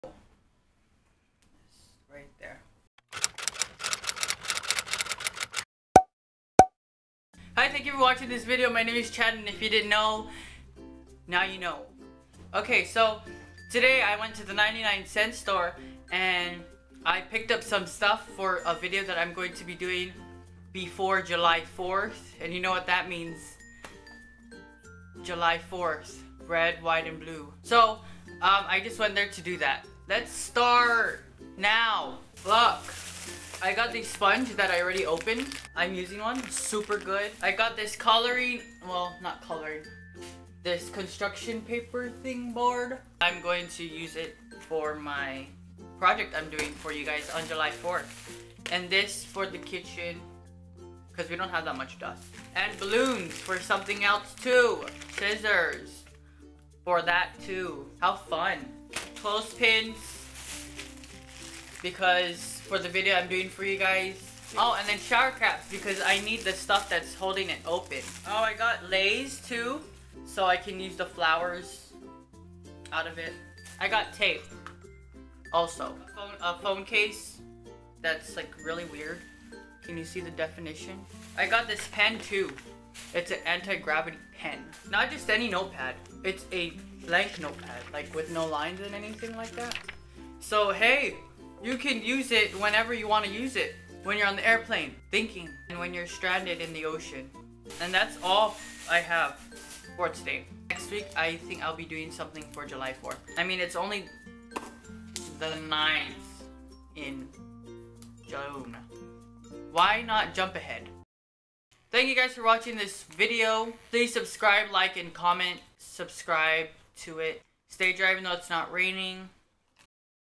99 cent store haul//Chad Salas//Friday
so i went to the 99 cent store to get some stuff that i will be using in a future video that i will be doing for july 4th and yes..hope you enjoy it.